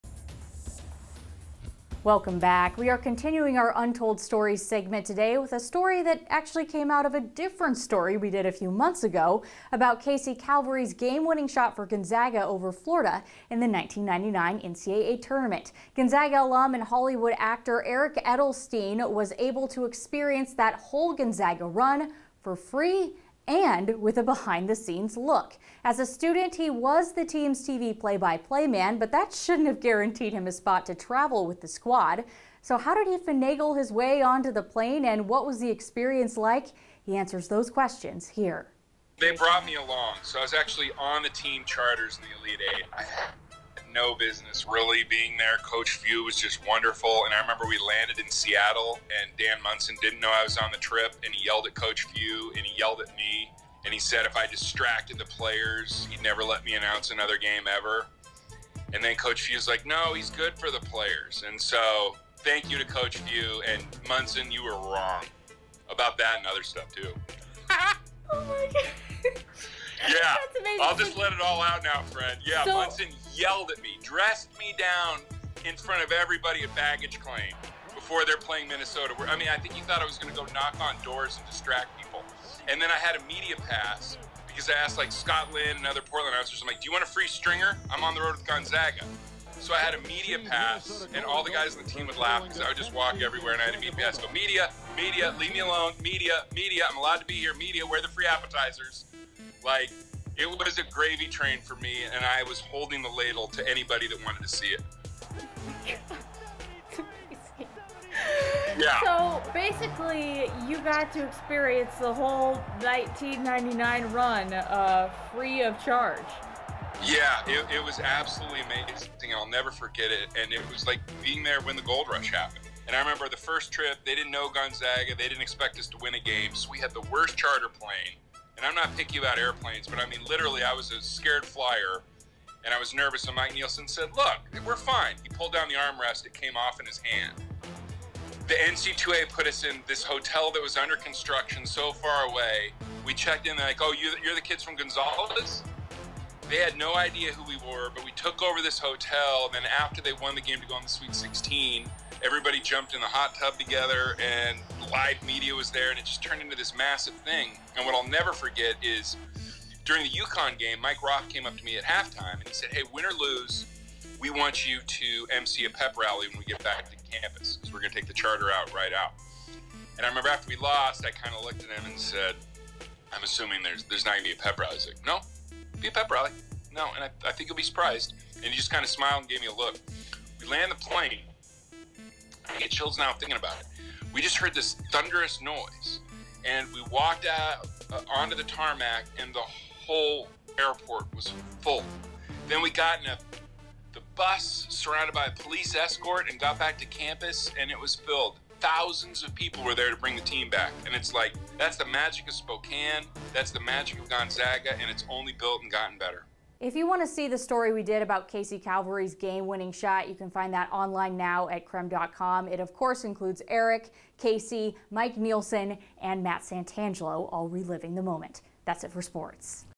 Untold Stories: How Gonzaga alum and Hollywood actor Eric Edelstein traveled with Gonzaga's team for
Times were a bit different for the Zags back then.